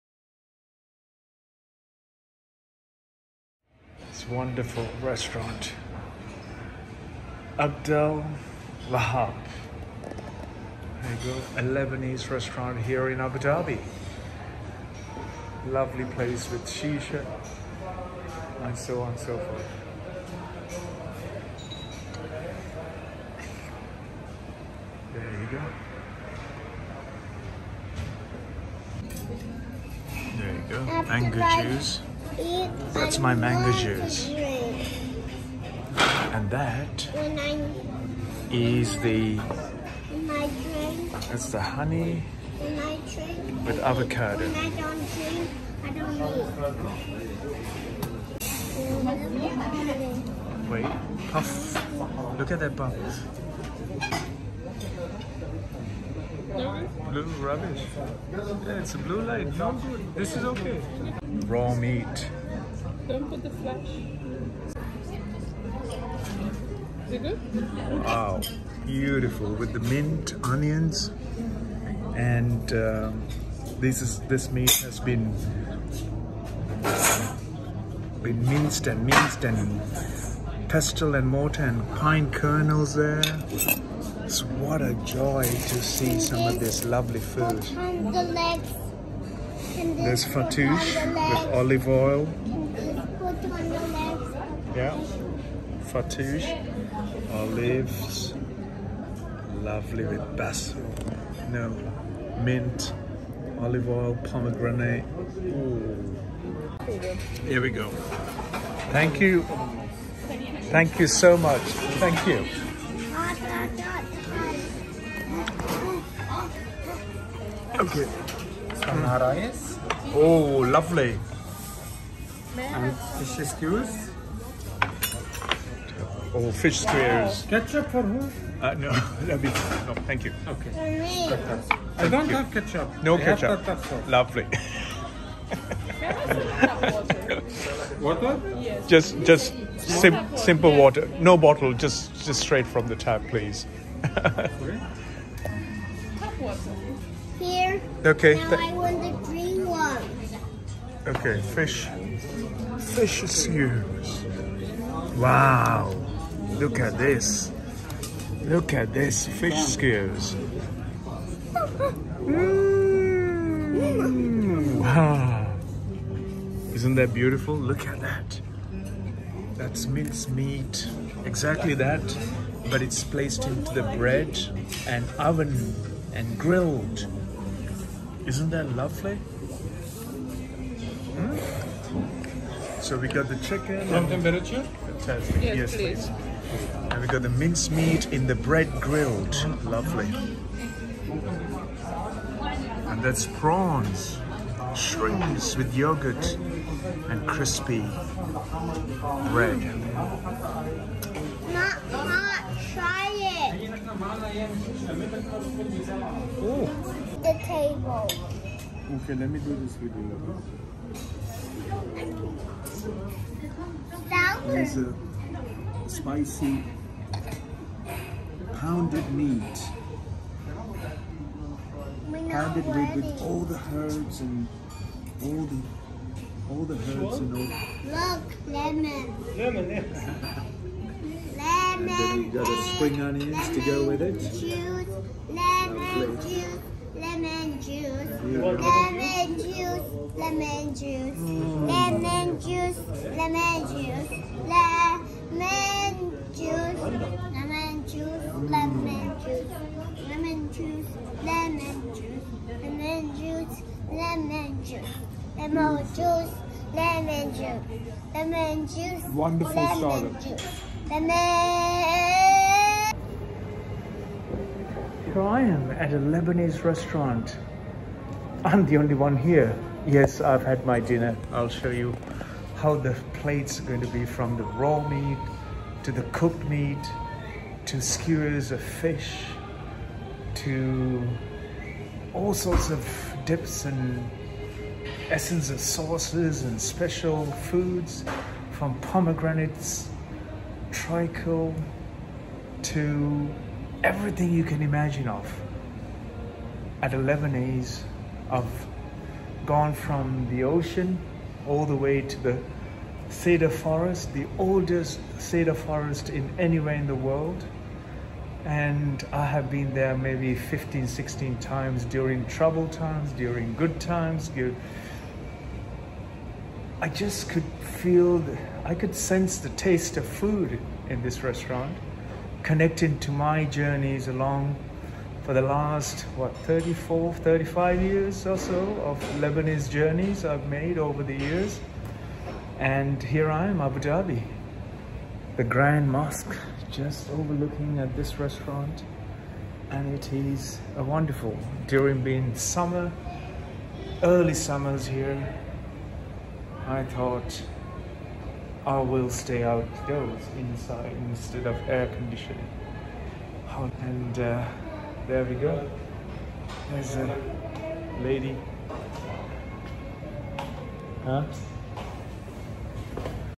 Lebanese dinner Abu Dhabi
Abd El Wahab Lebanese restaurant Souk Qaryat al Beri Abu Dhabi

Reflecting everything great about Lebanon and its culture, this restaurant embodied the genuine heart of the Lebanese people.